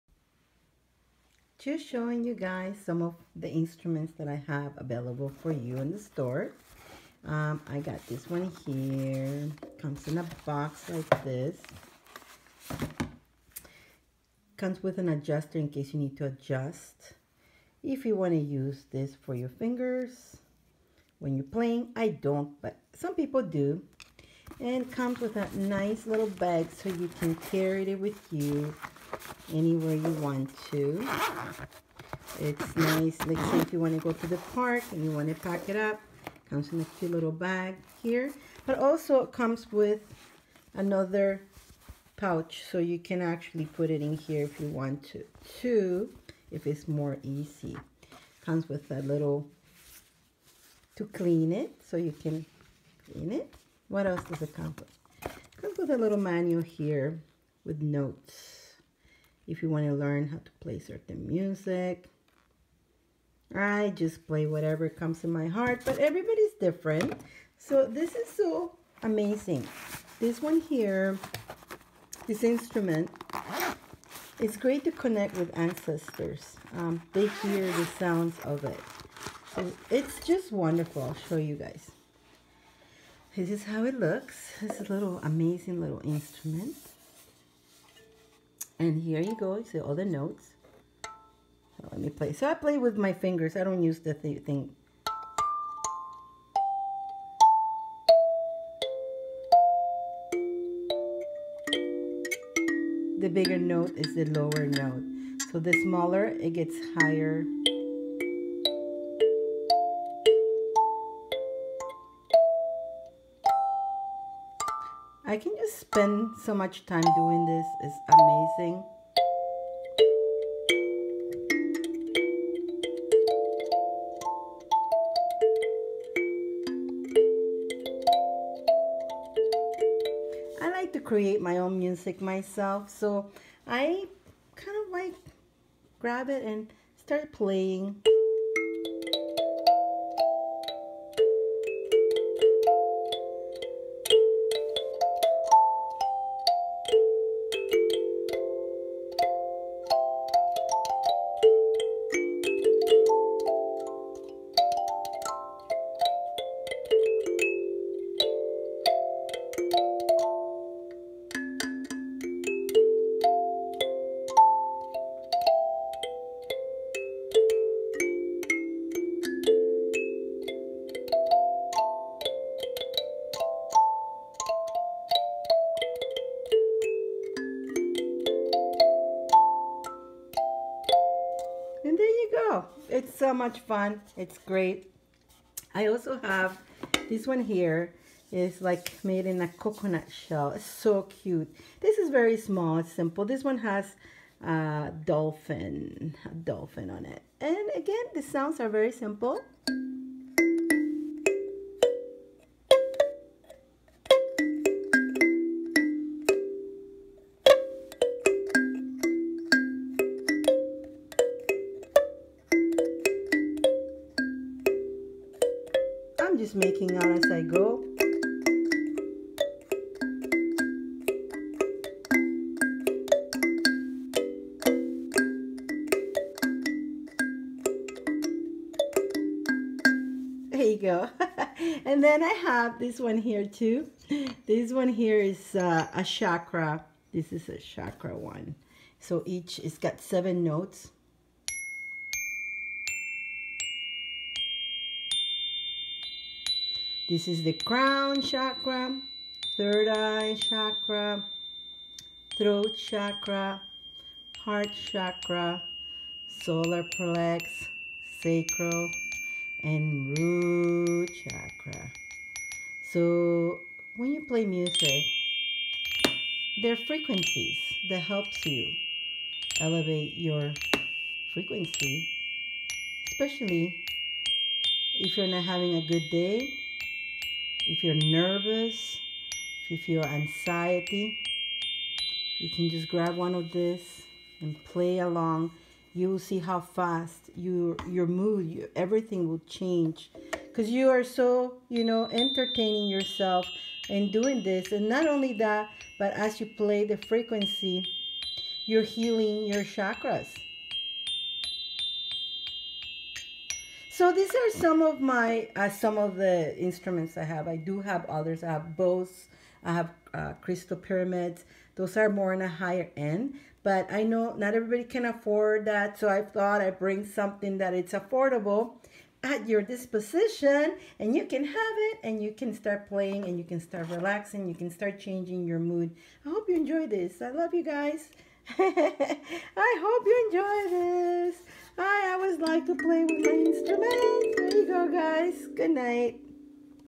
SHOWING MY KALIMBA AND OTHER POWERFUL INSTRUMENTS
THIS ARE SOME OF THE COOLES INSTRUMENTS TOOLS TO HAVE TO HELP YOU IN

YOUR HEALING OF YOUR CHAKRAS,
HELP YOU RELAX,
CALM DOWN YOUR ANXIETY,
ALSO ELEVATES YOUR FREQUENCY
 TAKES YOU OUT OF THE NEGATIVE ENERGY.
 AND WHEN YOU DO THAT, YOU BECOME MORE HAPPY
AND YOU WILL BE INSPIRED AND YOU MAY EVEN GET IDEAS TO CREATE THNGS YOU NEED TO CREATE IN LIFE. AND SO. MUCH MORE BENEFITS.